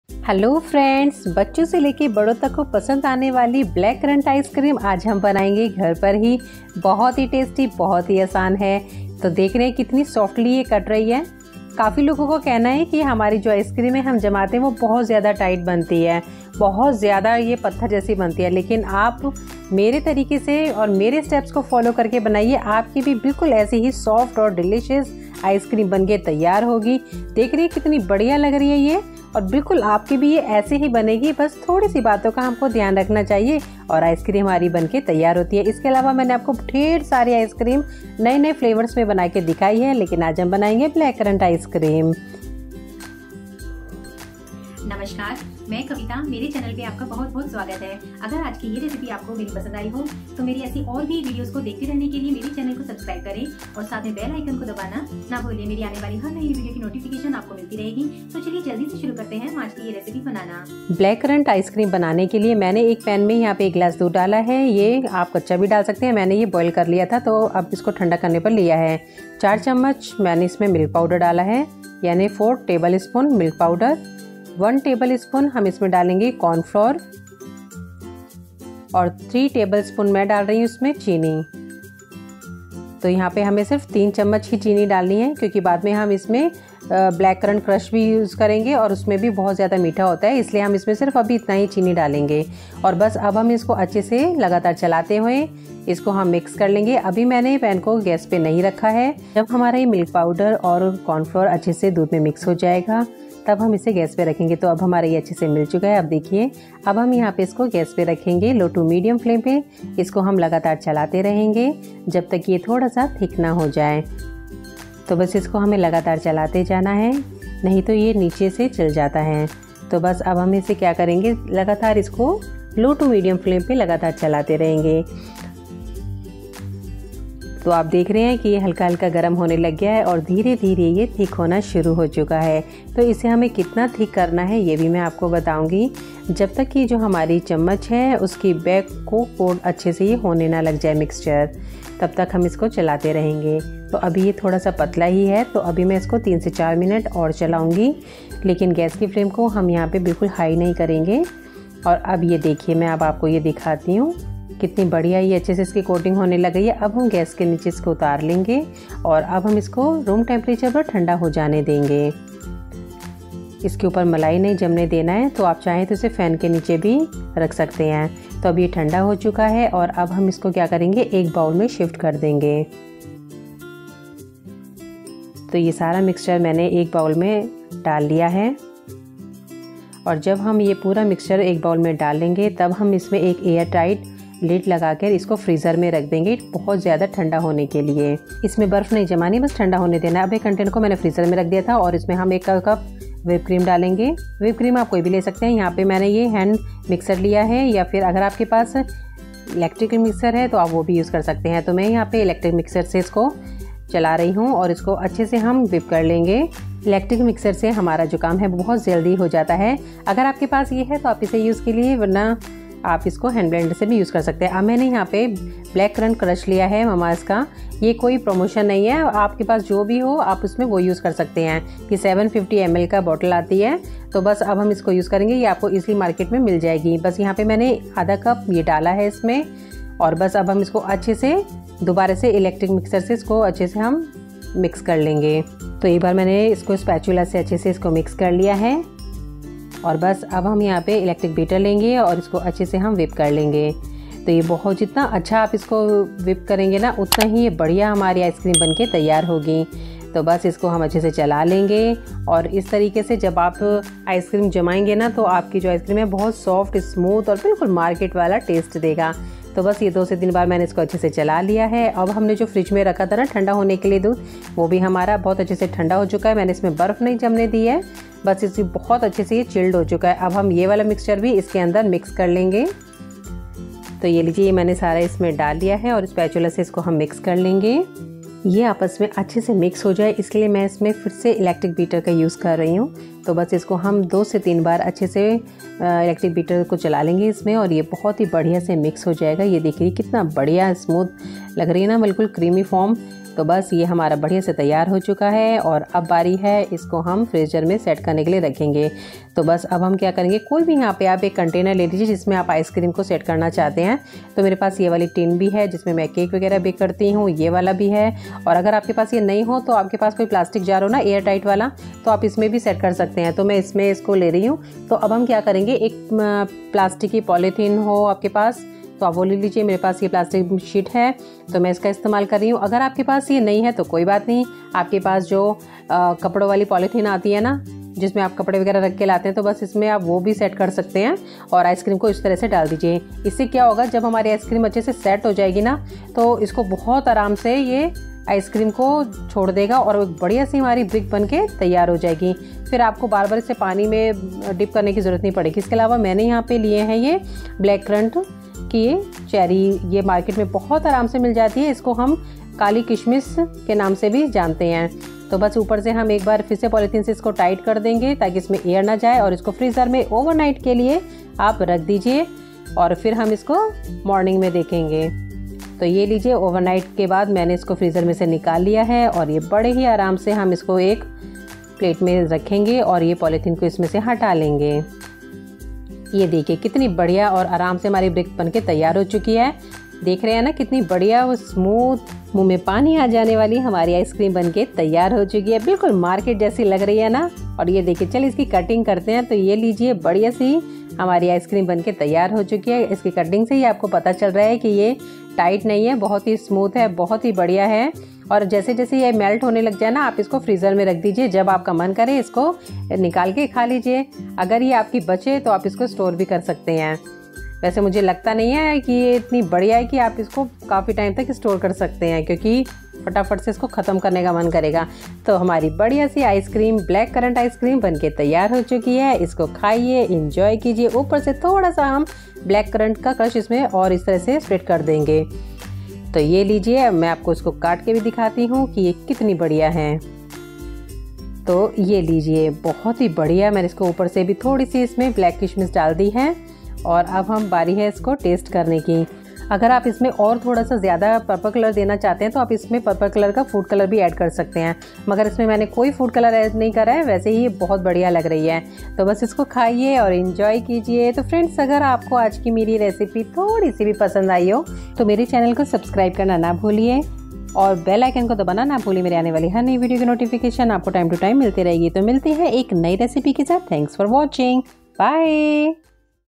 Black currant ice cream recipe | बाजार से भी अच्छी ब्लैक करन्ट आइसक्रीम | By-Kavita
Black currant ice cream recipe |
Ingredients -
Whipped cream - 1 cup
Black currant crush - 1 cup
Black currant
Follow me on your favourite social media channels:-
Important playlists that you should watch:-